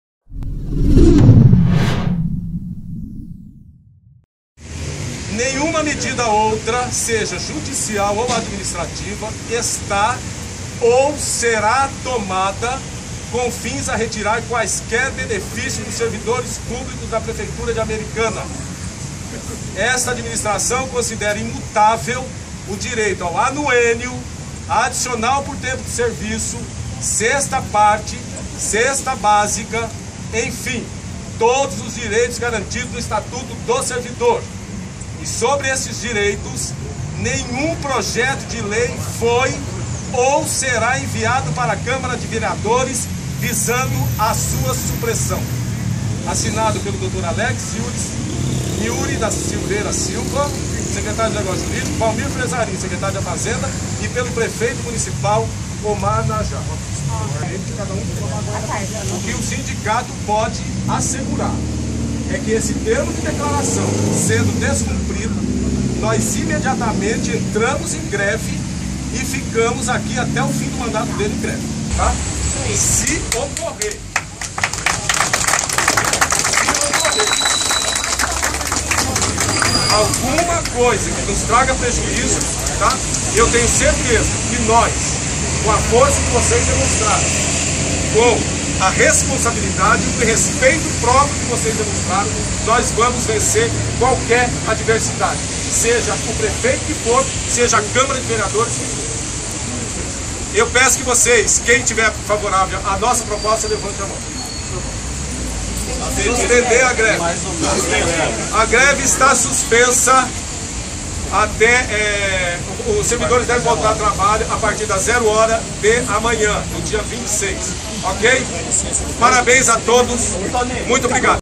Em Americana, greve acaba e Omar assina compromisso
Cesta básica terá alteração, mas será para punir os funcionários com falta injustificada por mais de 10 dias no mês